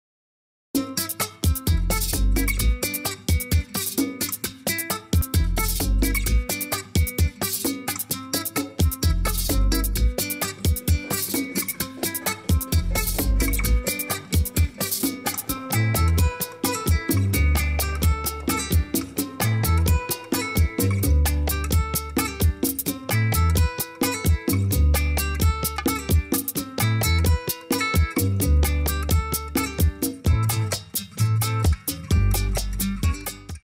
Bachata Grundschritte Dame mit Musik
Hier siehst du den Damengrundschritt für den wunderbaren Tanz "Bachata" Du willst mehr lernen? Begleite uns auf eine Tanzreise!